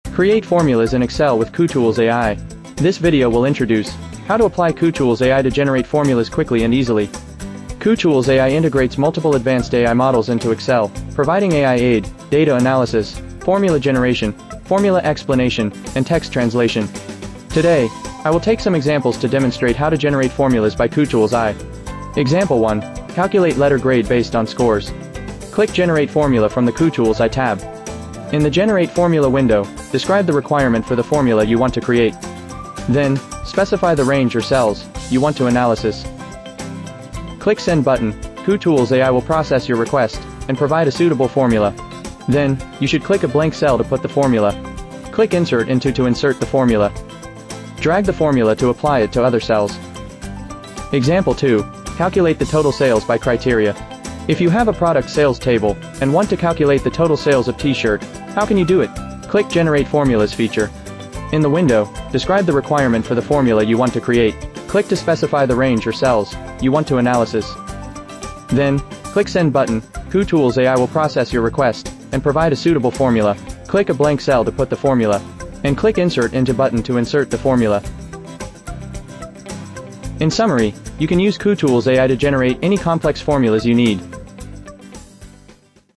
Excel formulas too complex? Kutools AI generates them with just one command!
From complex to simple! Kutools AI transforms your words into flawless Excel formulas in seconds. The future of spreadsheet automation is here!